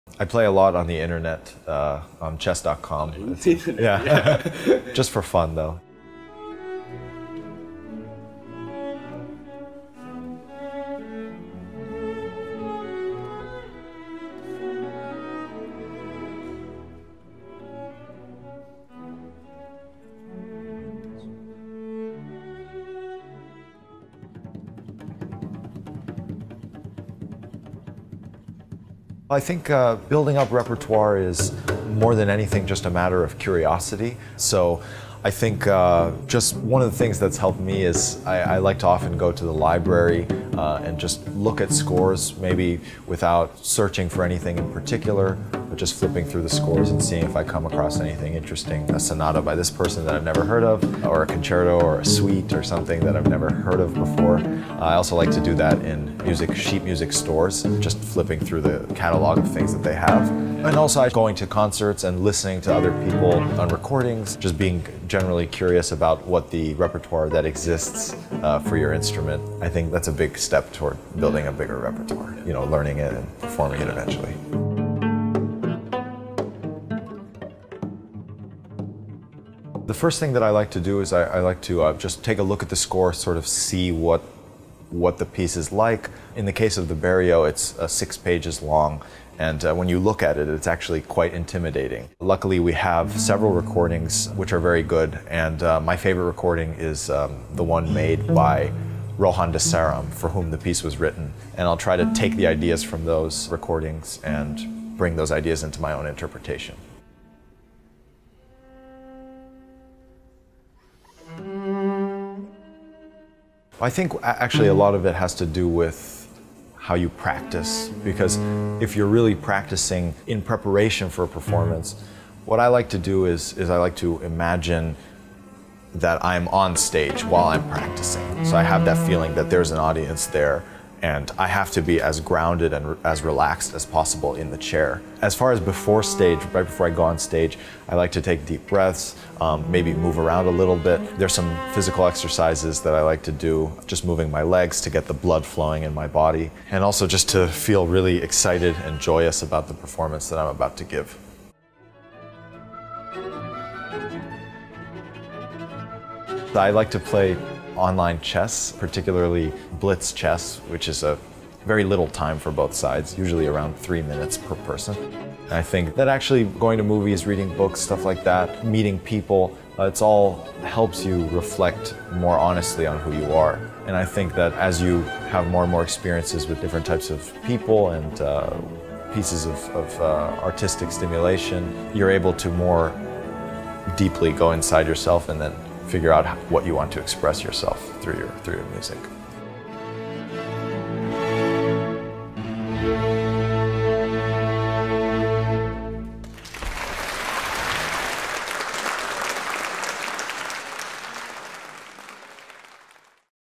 [ 大師悄悄話 episode 15 ] 馮勇智, 大提琴家 Zlatomir Fung , Cellist
大提琴家 馮勇智 Zlatomir Fung

「藉由音樂探索自己。」─ 馮勇智

2019柴科夫斯基國際音樂大賽首獎得主，大提琴家馮勇智來到muzik啦！一路過關斬將的他，是如何鞭策自己以及保持對音樂的熱忱呢？年僅20出頭的馮勇智，又會為我們帶來什麼悄悄話呢？

/ MUZIK 大師悄悄話 /  紀錄弦外之音  分享吉光片羽

歷練成就人生，也豐富藝術
大師不藏私，擴張你的音樂想像

每個月 15 號晚間 8 點，準時上線！